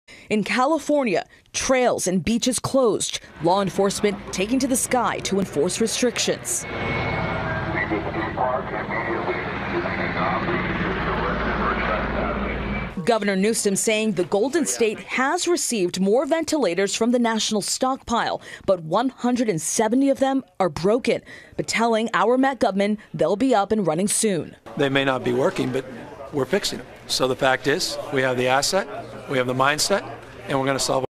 What I As California Is Doing Within My Borders Is Doing Due To The Coronavirus (COVID-19)
All Public Places Are Closed Within I As California